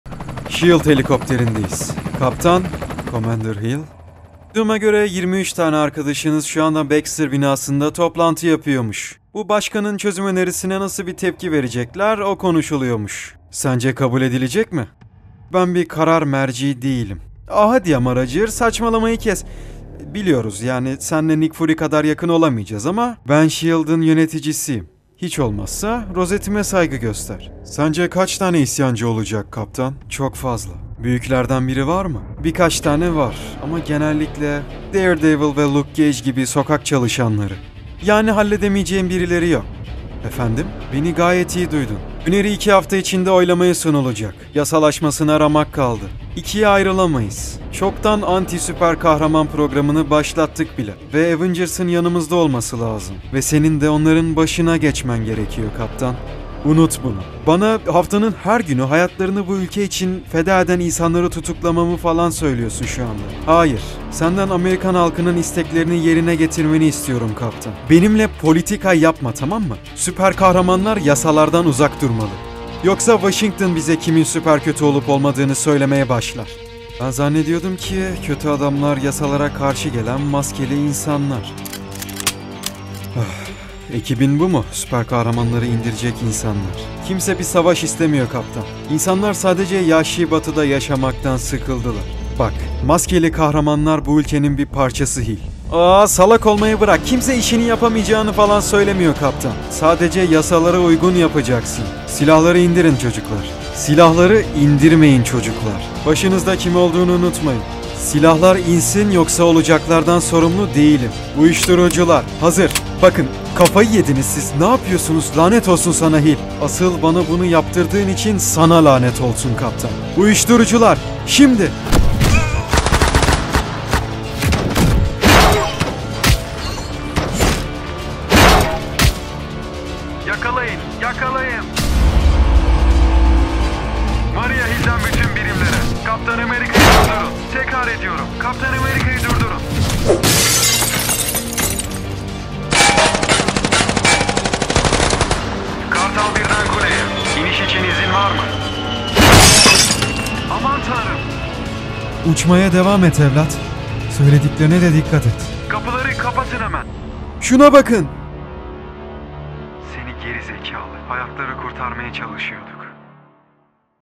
Kaptan Amerika S.H.I.E.L.D.'a Karşı
Kaptan Amerika  bir karar vermek zorunda ,kahraman yasasını kabul mu etmeli yoksa yer altına mı inmeli ? Evet dostlarım uzun İç Savaş videosundan bir sekans. Sizleri boş bırakmamak adına klip niteliğinde bir video paylaşıyorum. Video 50-60 dakika uzunluğunda olacak ve beklediğinize değecek. Takipte kalın seviliyorsunuz !